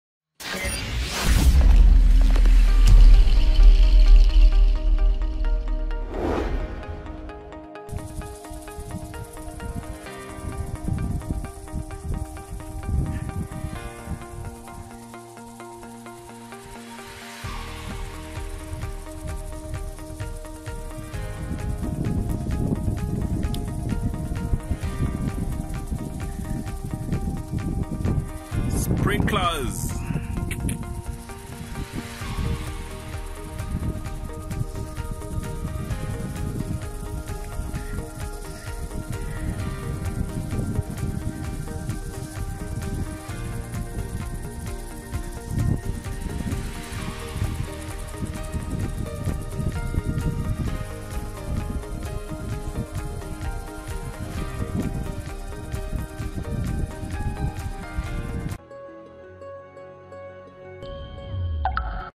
Béc tưới cánh đập Automat phi 27 - AQ46 (Việt An Nông)
Bán kính: 10 - 14m, lưu lương: 1 - 1.5m3/h, áp suất: 1 - 3 bar. Bảo hành 18 tháng.